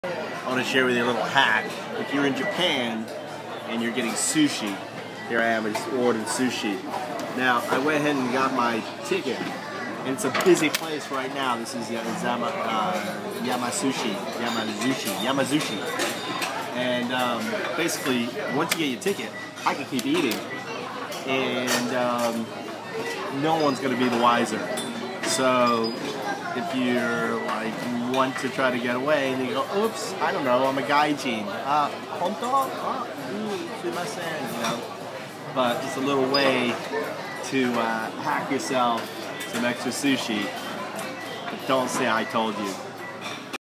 How to hack free sushi in japan
How to hack some free sushi :) I am just showing a problem of the current HamaIshi fast food system. I do not condone doing this.